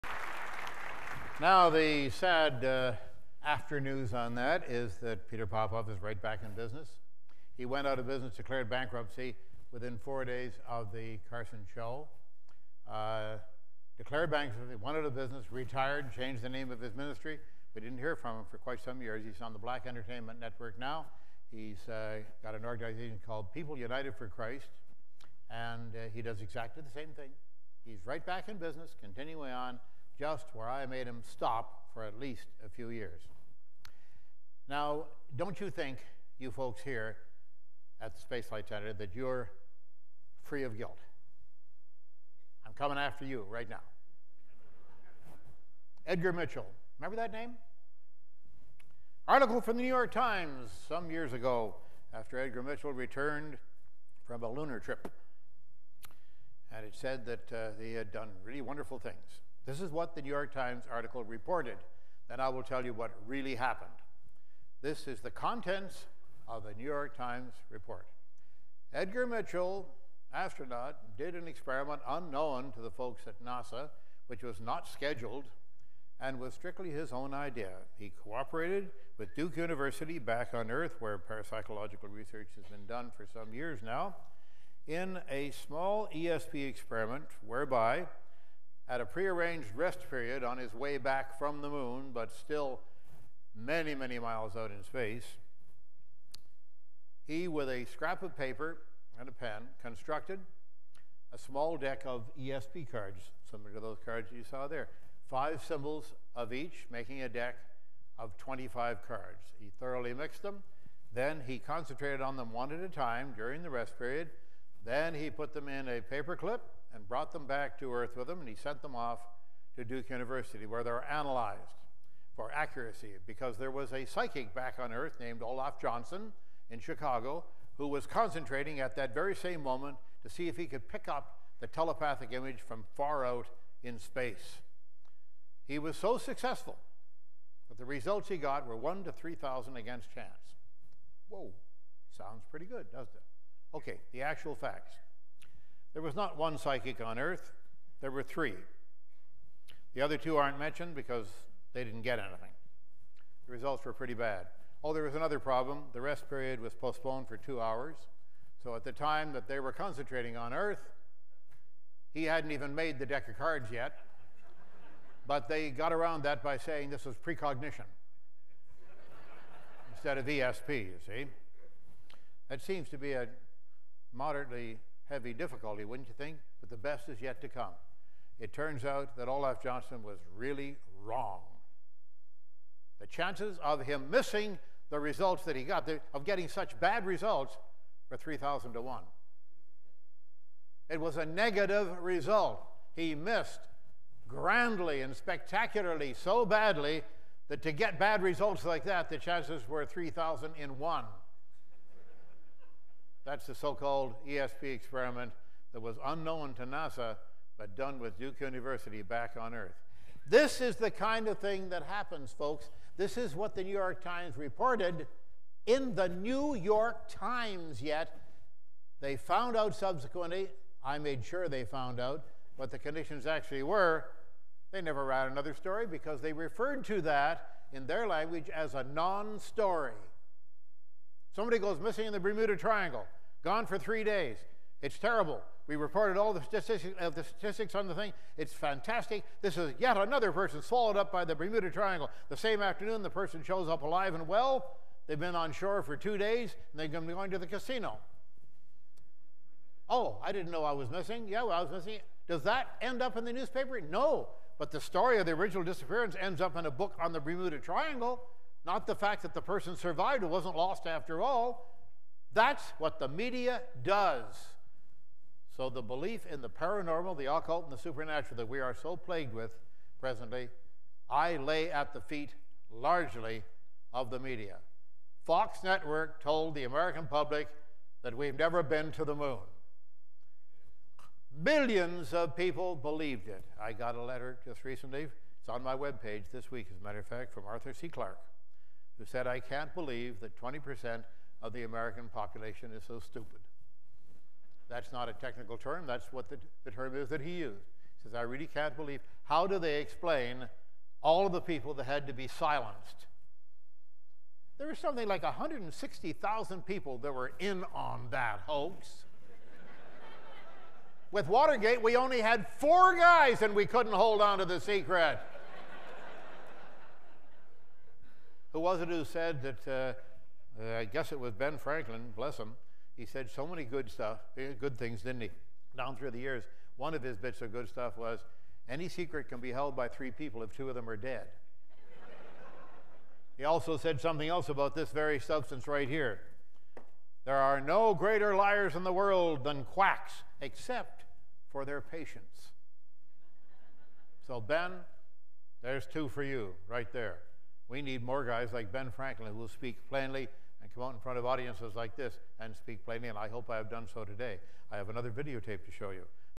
James Randi Lecture at NASA 10/12
JREF DVD332
Full James Randi lecture at NASA. Randi talks about all things woo, the million dollar challenge, and homeopathy.